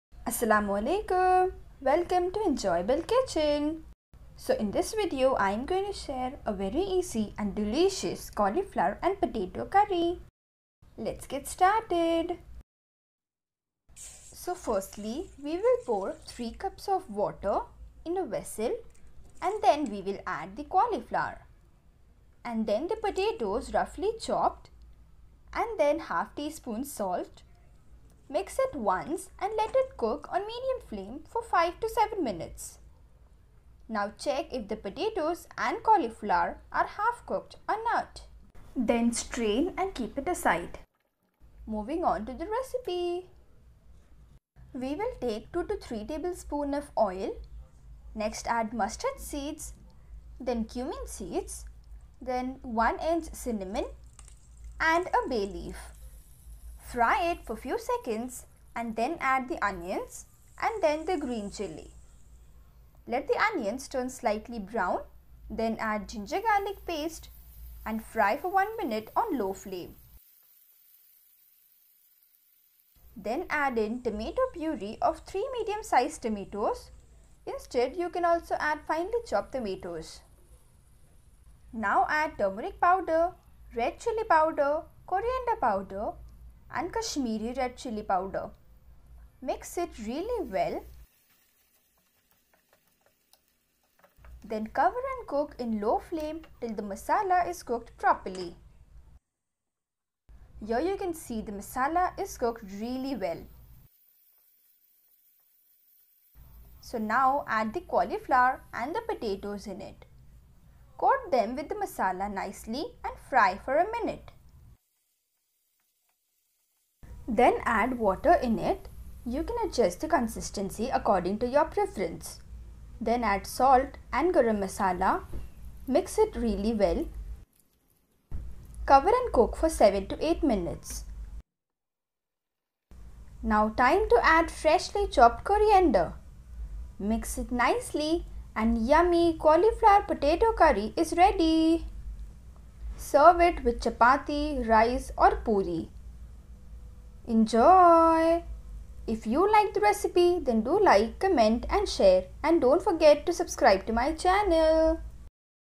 Cauliflower And Potato Curry | Aloo Gobi Recipe | How To Make Cauliflower And Potato Curry
And amazing recipe which you can serve with Chapati, Rice or Poori

Ingredients :

FIRST STEP :
Cauliflower - 250 gm
Potato - 3 medium size (Roughly Chopped)

SECOND STEP :
2-3 tbsp Oil
1 tsp Mustard Seeds ( Rai)
1 tsp Cumin seeds (Jeera)
1 inch Cinnamon (Dalchini)
1 Bay leaf
2 medium size Onion ( Finely Chopped)
1 Green Chilli (Finely Chopped)
1 tbsp of Ginger Garlic Paste
3 medium size Tomatoes (Tomato Puree /Finely Chopped)
1/4 tsp Turmeric Powder (Haldi powder)
1 tsp Red Chilli Powder
2 tsp Coriander Powder (Dhania Powder)
1 tsp Kashmiri Red Chilli Powder
1 tsp Garam masala
Freshly Chopped Coriander

If you liked the video then